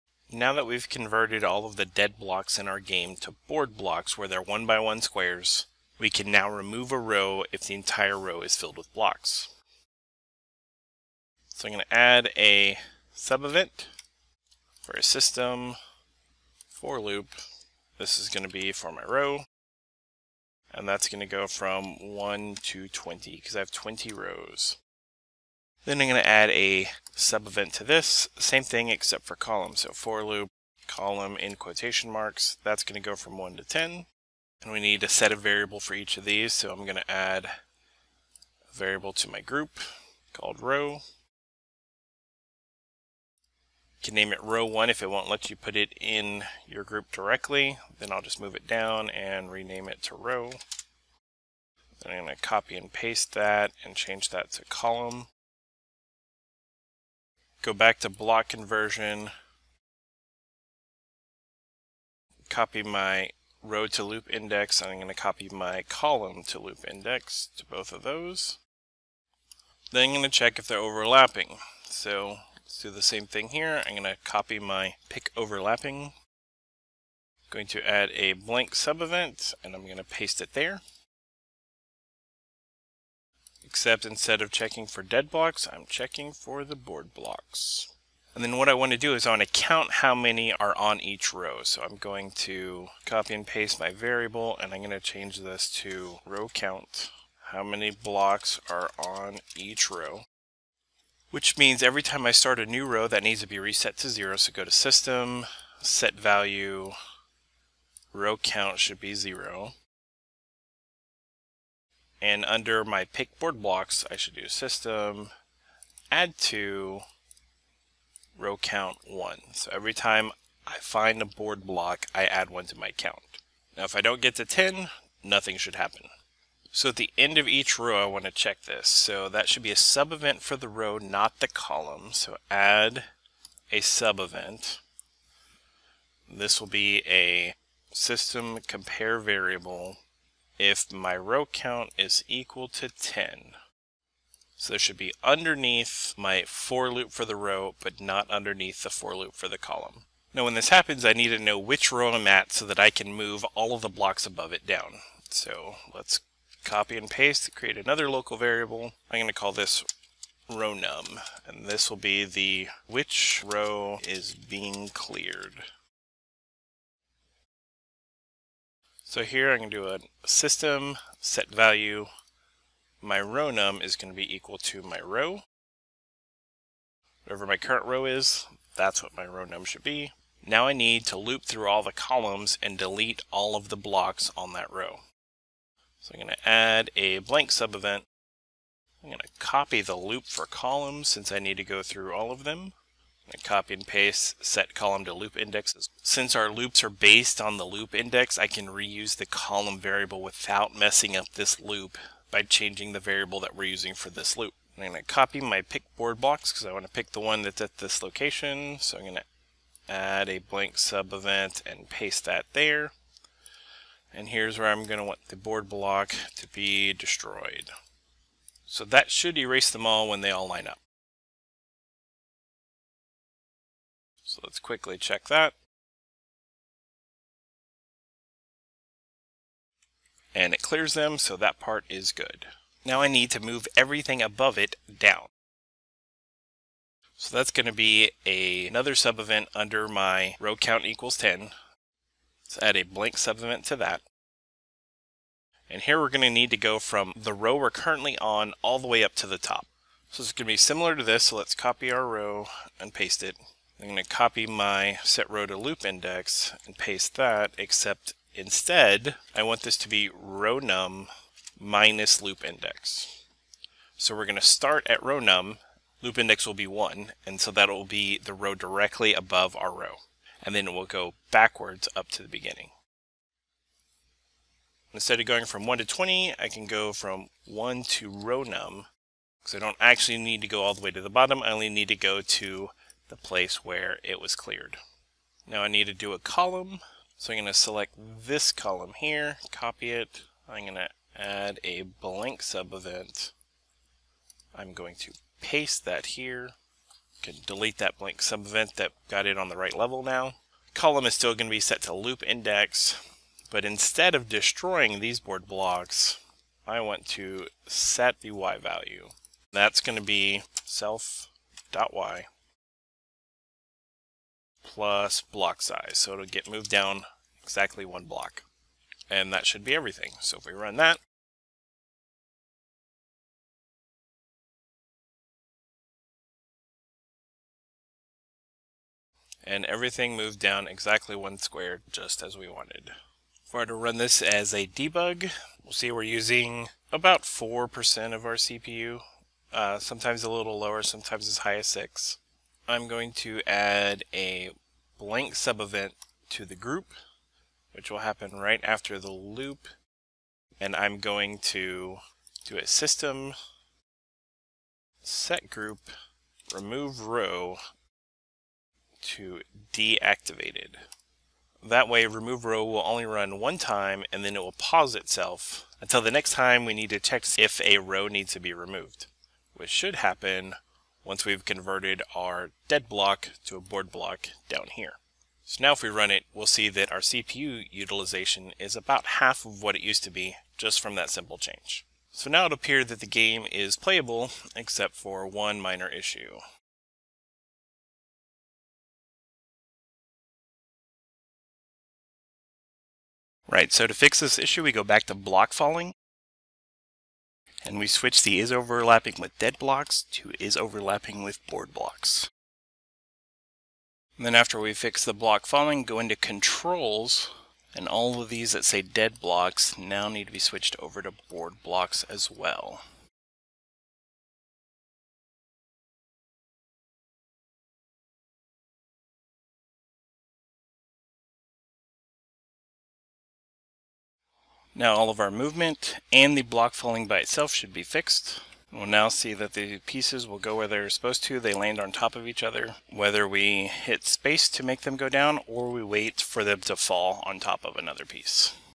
Tetris - Construct 2 - Part 5 - Clear A Row
In this lesson we search for a low with 10 blocks in it, and then remove everything in the row when we find one. Then we move up and move each block that we find, down one block.